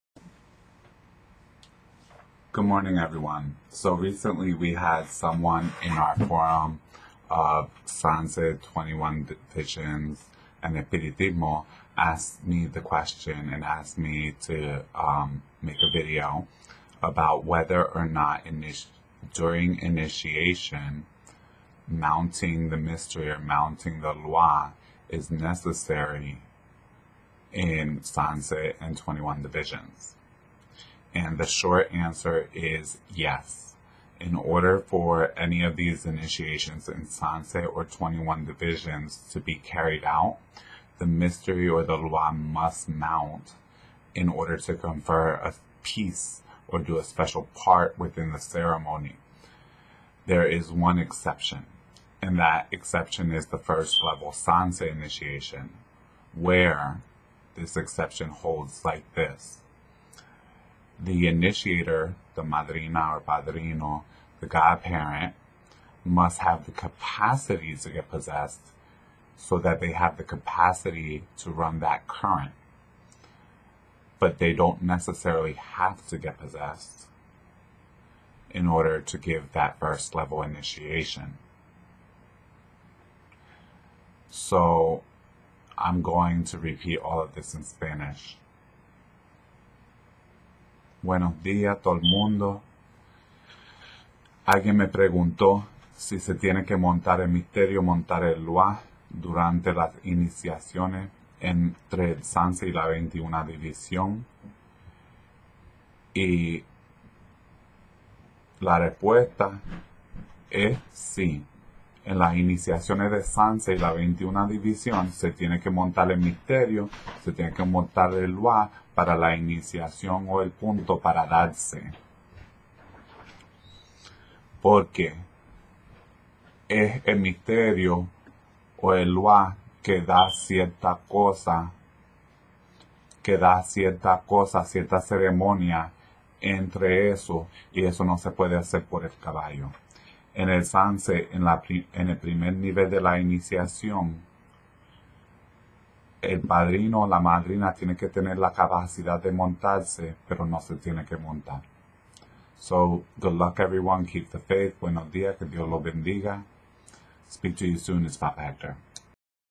Initiation Sanse 21 Divisiones- to mount or not
Papa Houngan Hector Salva discusses whether or not mounting the Misterio, Mystery or Lwa is necessary during initiation ceremonies with Sanse and Las 21 Divisiones.